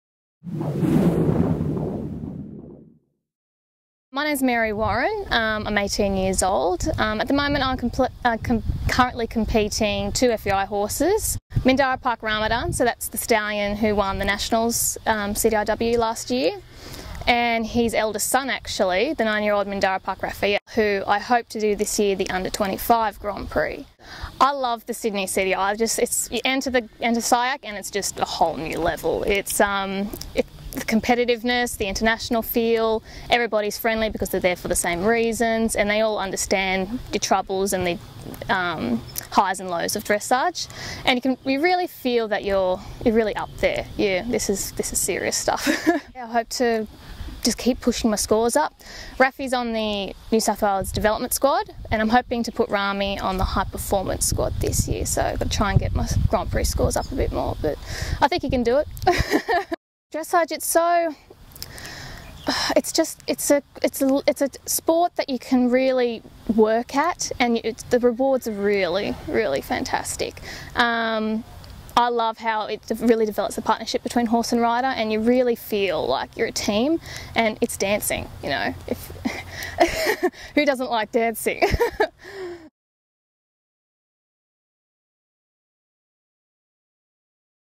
Mary Warren Interview
Young Rider Mary Warren talks about her plans for the 2017 Sydney CDI